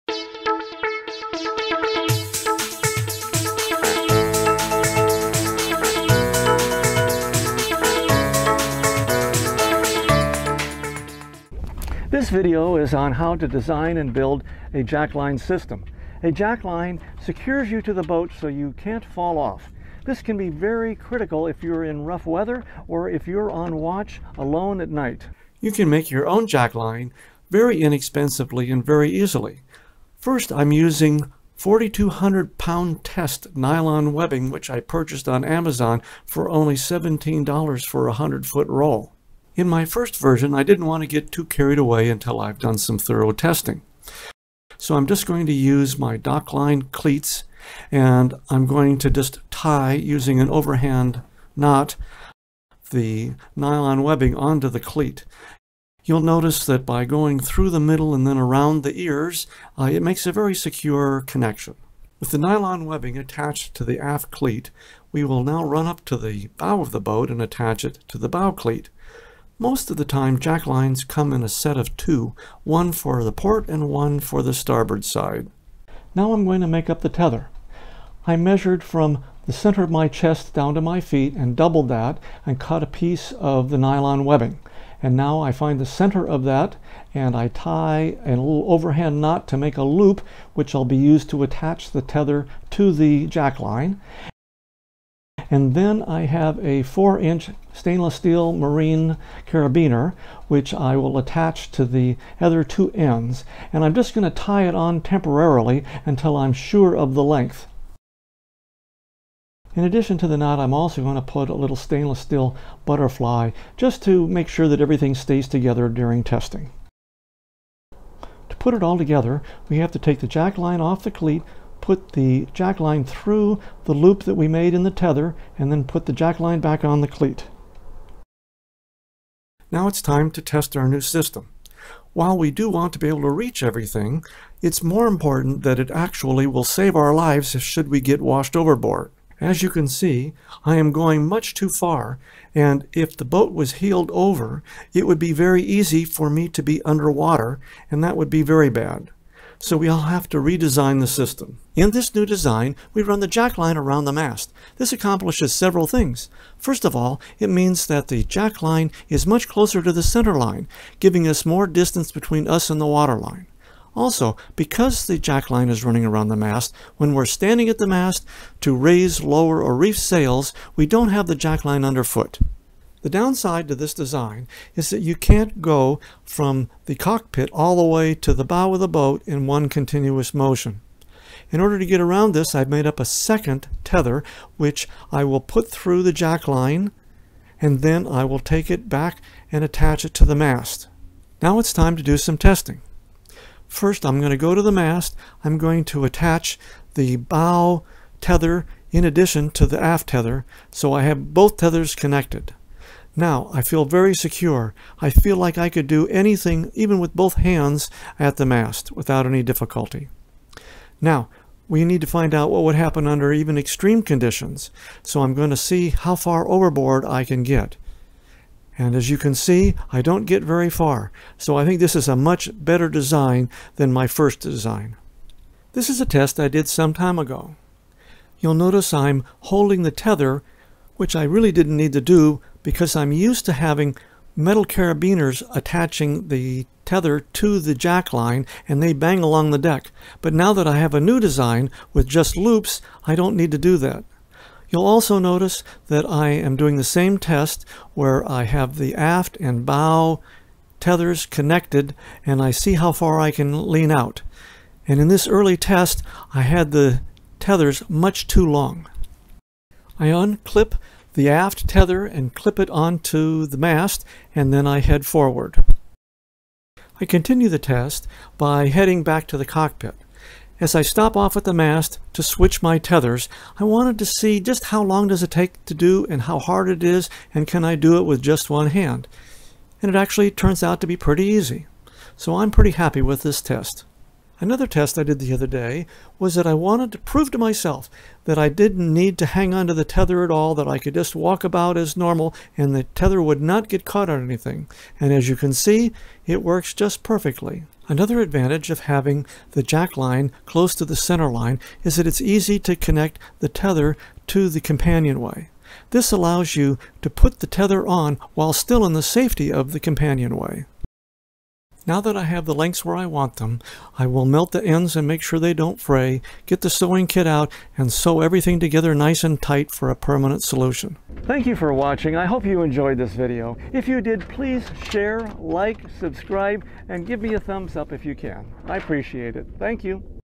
Jackline Redesigned
This is a new video that replaces the two older jackline videos. It is a much better redesign. I no longer use any plastic clips and it is ever less expensive and easier to build. Using 4,200 lbs 1 inch nylon webbing this jackline should hold in the most severe conditions. A Jackline is suppose to keep you on the boat and I believe this design will accomplish this with minimal fuss. While every boat is different I hope the ideas presented here will help you design a jackline the works for you.